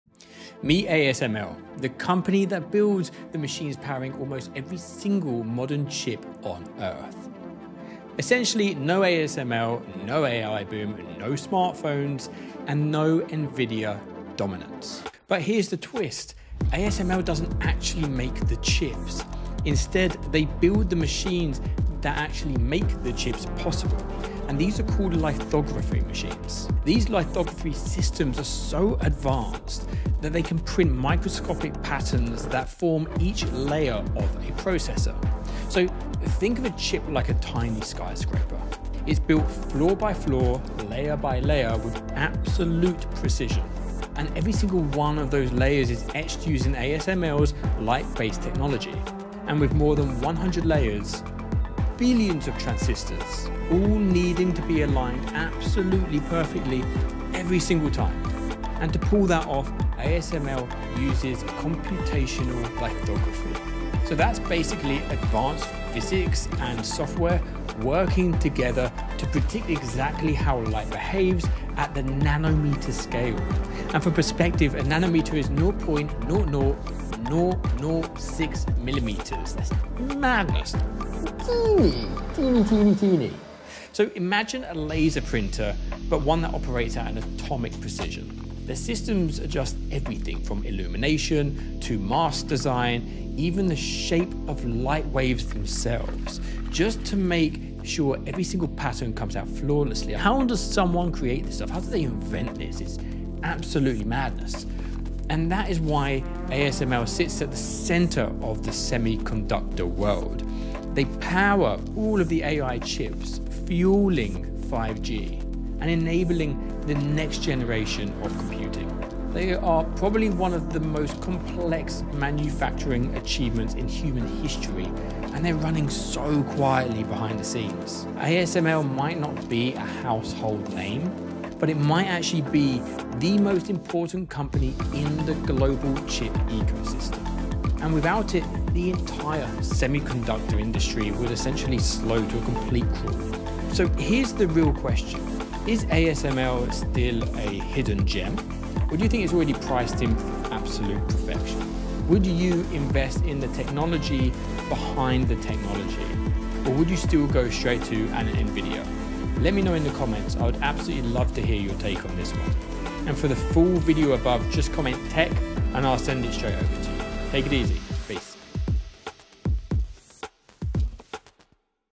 The Secret Company Behind Every AI Chip
ASML is the company powering almost every advanced chip on Earth — from AI processors to smartphones. They don’t make chips… they make the machines that make chips possible. In this video, I break down how ASML’s lithography systems print microscopic patterns with atomic-level precision and why the entire semiconductor industry depends on them.

🎯 In this video, you’ll learn:
➡️ How ASML’s EUV technology works
➡️ Why their machines are nearly impossible to copy

Comment TECH to get the full video.